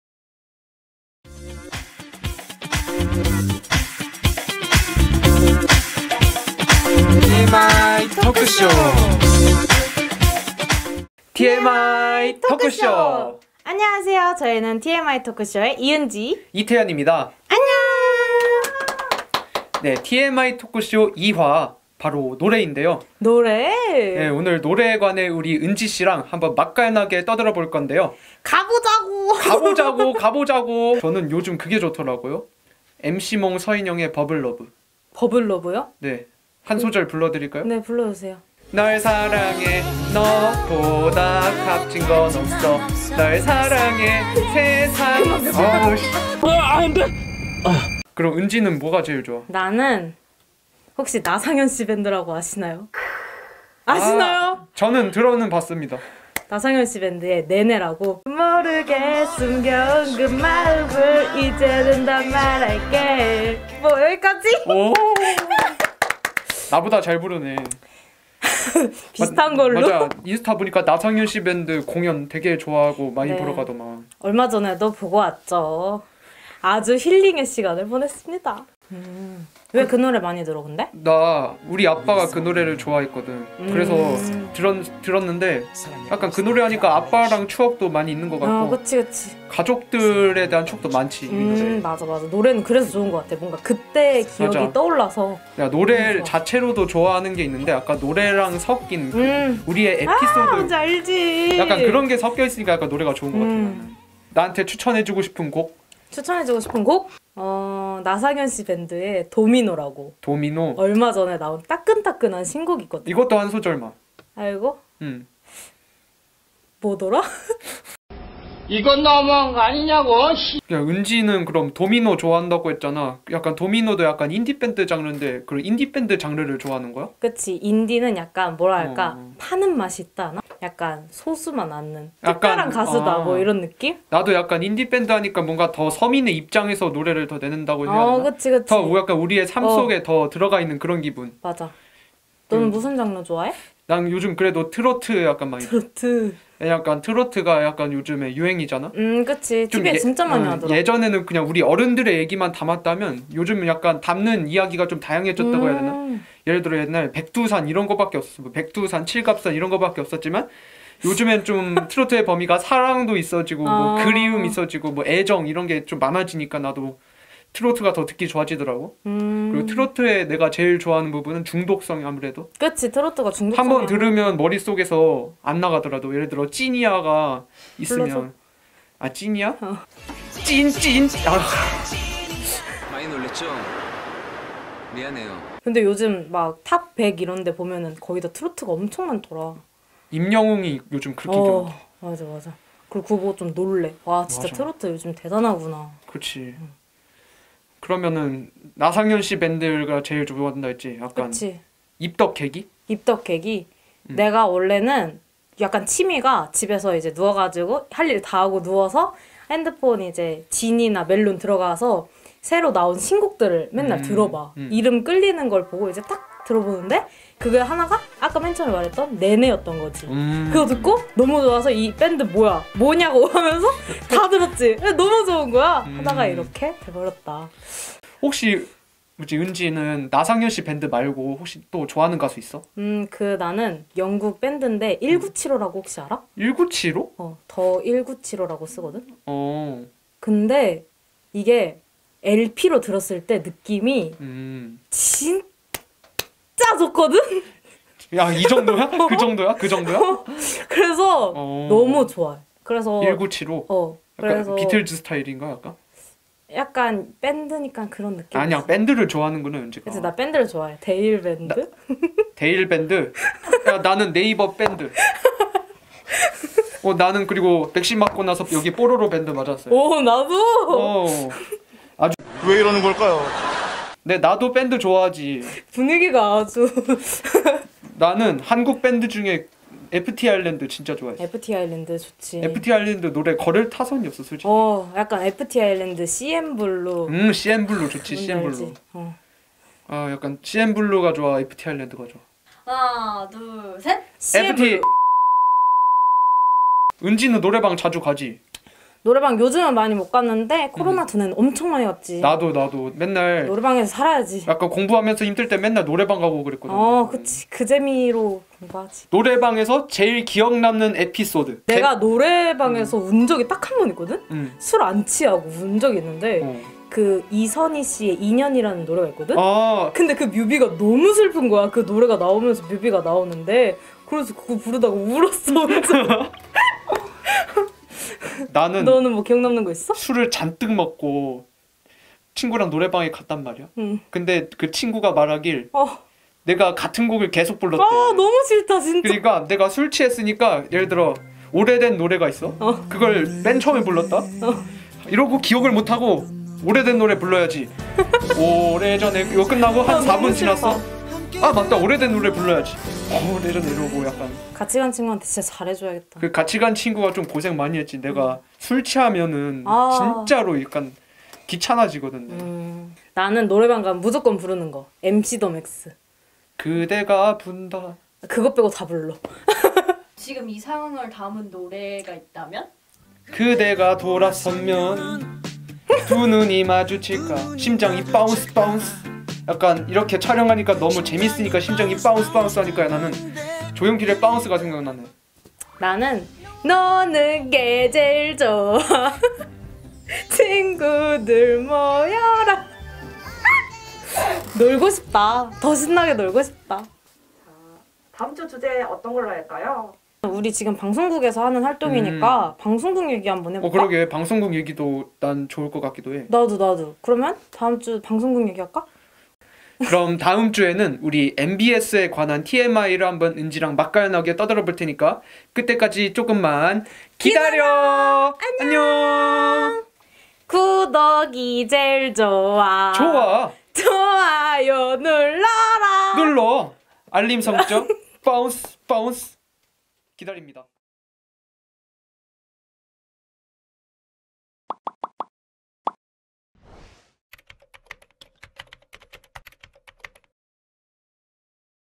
[EP2 노래 | TMI 토크쇼]
EP2 TMI 토크쇼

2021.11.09

마치 친구와 이야기하듯이 가벼운 이야기들을 풀어놓을 수 있는 방송
매주 다른 아무 말로 찾아오겠습니다!
이번주 주제는 '노래'입니다.

제작 신유영 기술 이영채 출연 이은지 이태연